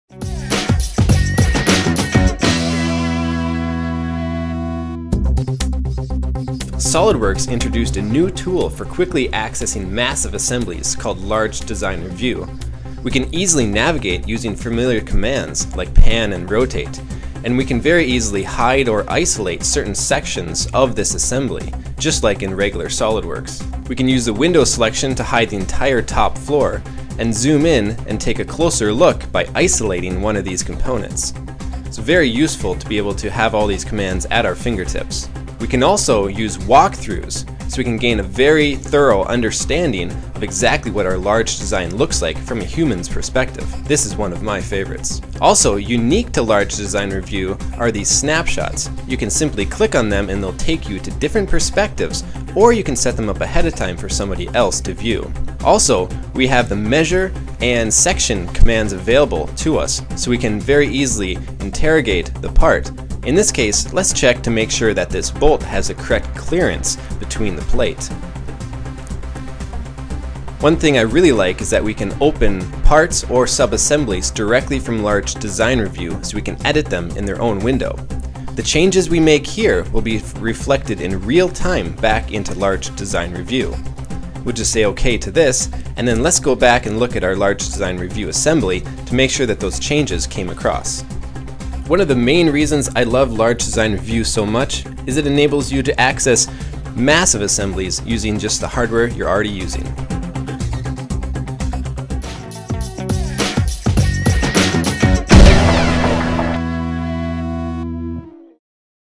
Large Design Review Mode in SolidWorks
The new Large Design Review with SolidWorks® Premium 2012 lets you open massive assemblies in seconds without the need for a high-end workstation. The view is fully interactive, just like working on a smaller assembly. You can explore, analyze, and then directly open the design to make changes.